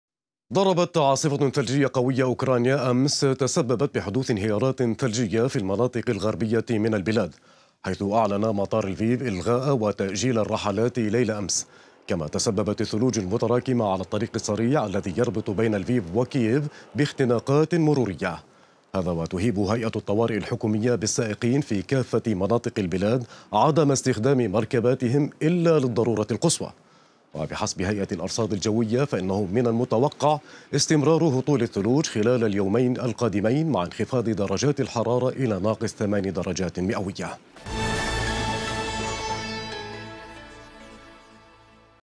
snعاصفة ثلجية قوية تضرب أوكرانيا ow
ضربت عاصفة ثلجية قوية أوكرانيا أمس تسببت بحدوث انهيارات ثلجية في المناطق الغربية من البلاد ، حيث أعلن مطار لفيف إلغاء وتأجيل الرحلات ليل أمس ، كما تسببت الثلوج المتراكمة على الطريق السريع الذي يربط بين لفيف وكييف باختناقات مرورية .